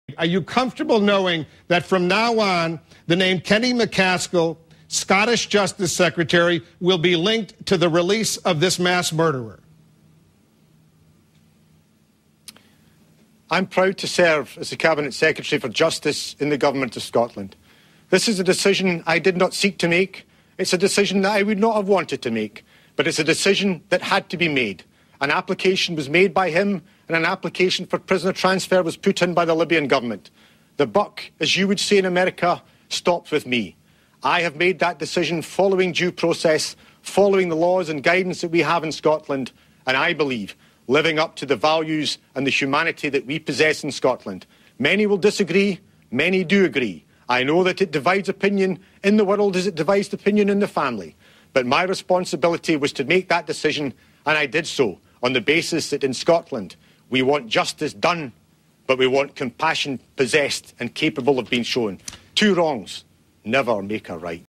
THE SCOTTISH JUSTICE MINISTER ON THE FREEING OF THE LOCKERBIE TERRORIST.
The Scottish Justice Secretary, Kenny MacAskill explains the reasons for freeing a terrorist who was convicted for his role in the bombing of Pan Am Flight 103.